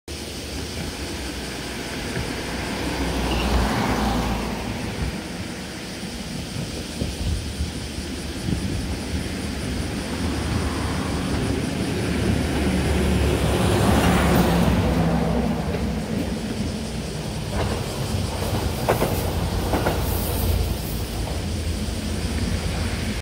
2023/8/7 普通列車(キハ220)走行@豊後三芳～日田間
2023/8/7　JR久大本線豊後三芳～日田間を走行する久留米行き普通列車(キハ220_1両)を撮影しました。